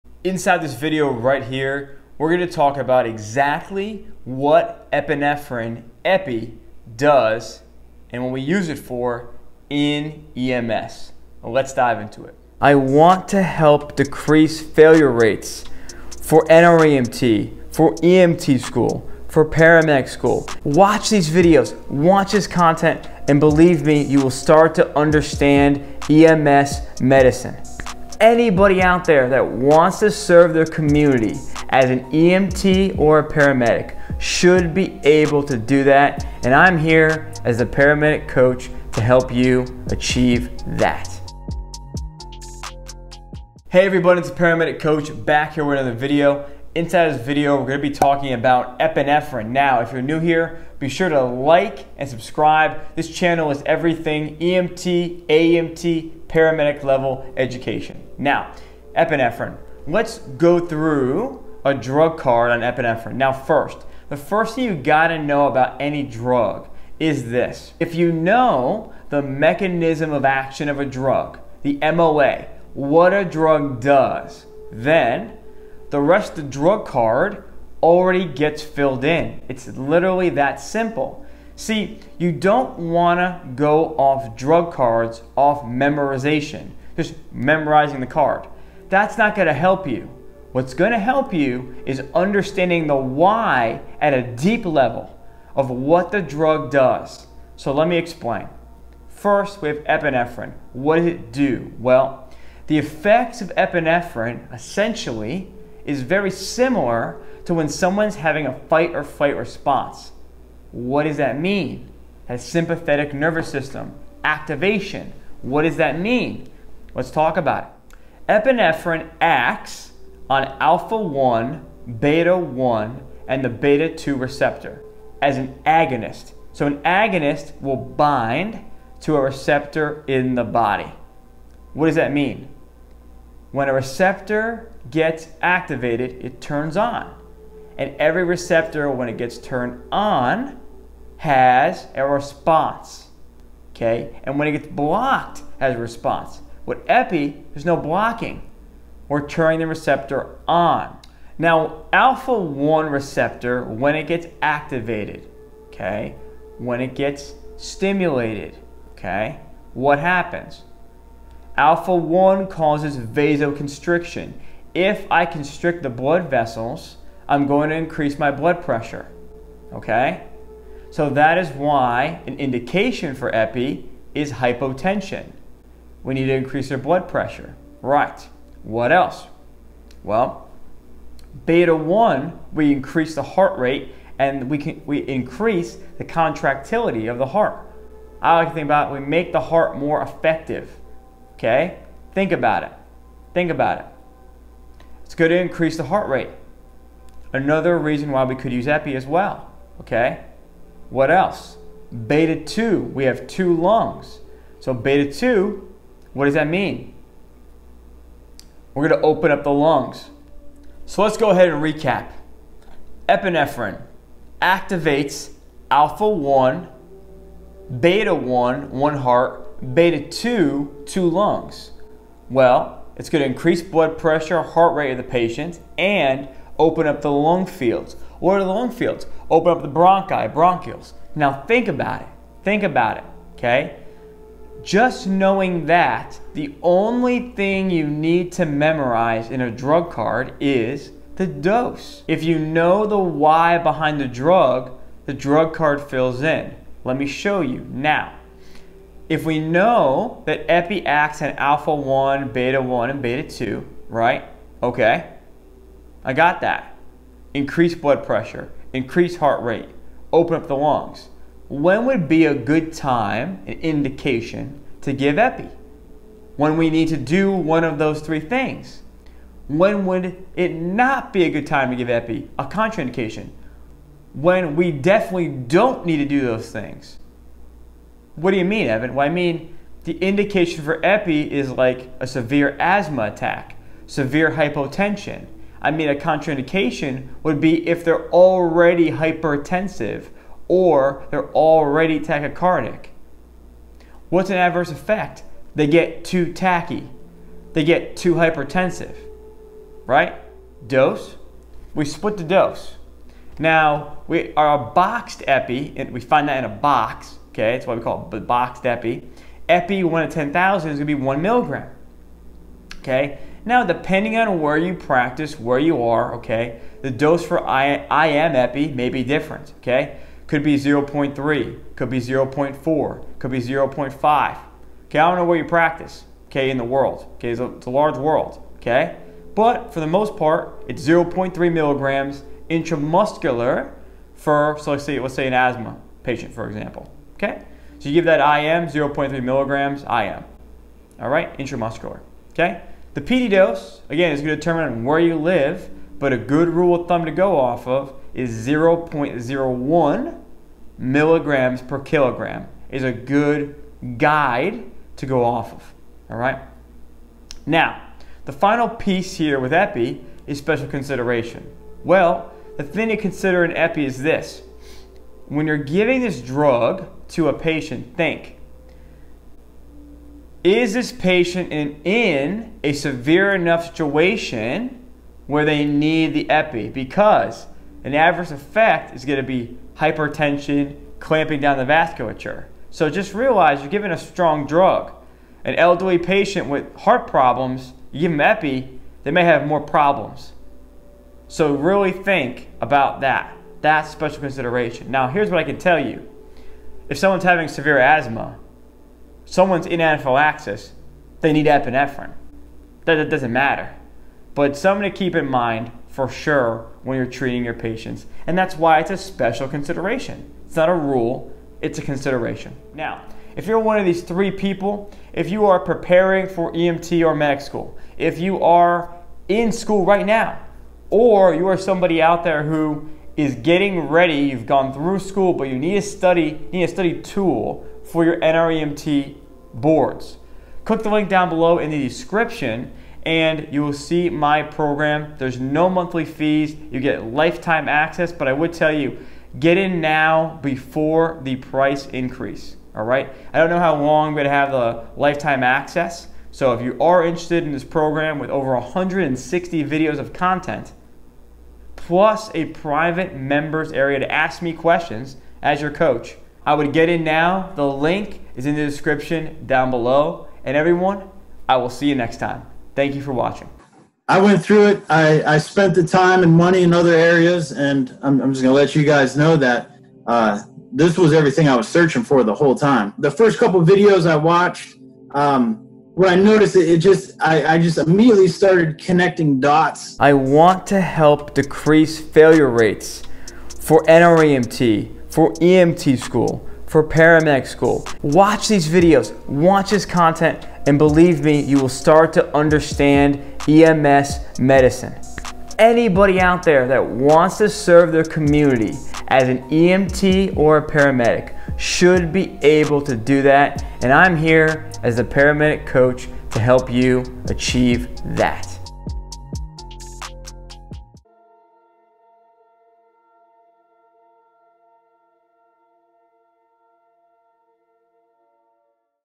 Epinephrine Use In EMS (EPI IN EMS EXPLAINED SIMPLY)
What is epinephrine? How does epinephrine work? Why do we give epinephrine in ems? Do EMTs give EPI-Pens? How to use an EPI PEN?

Let's answer some of these common questions and fully understand the entire "drug card" of epinephrine so we can use it successfully and confidently out in the field.